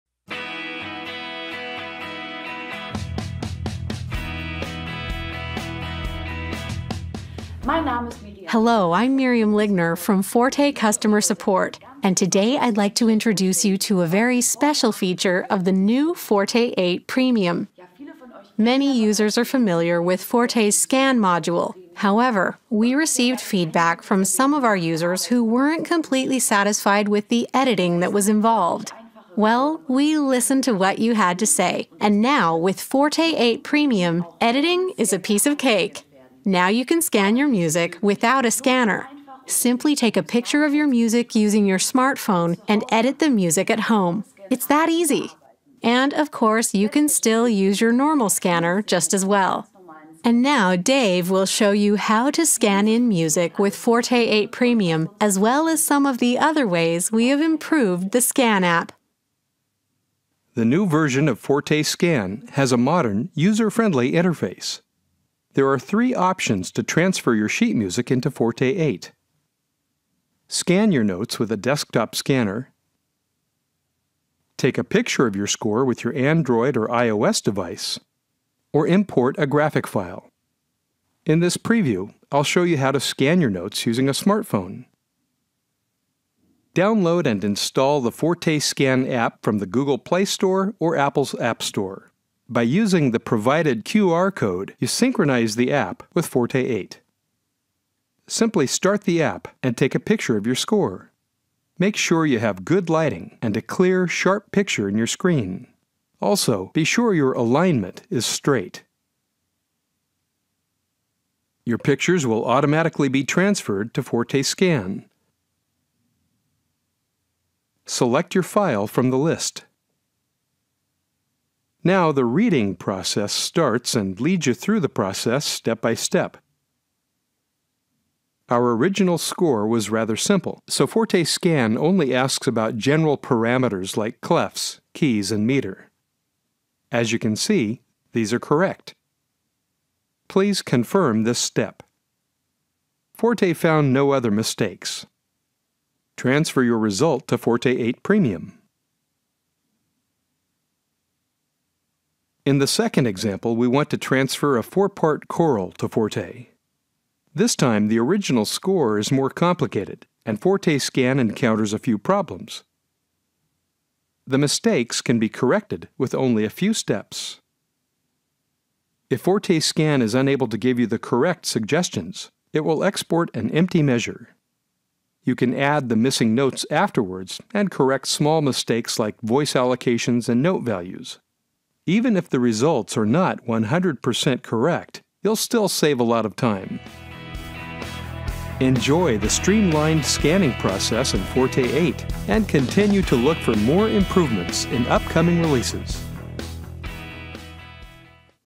Preview FORTE 8: The New Scan Module
In this video we will show you the new scan module of FORTE 8 Premium. It allows you to scan your sheet music with a mobile android or iOS device. Enjoy scoring with FORTE 8.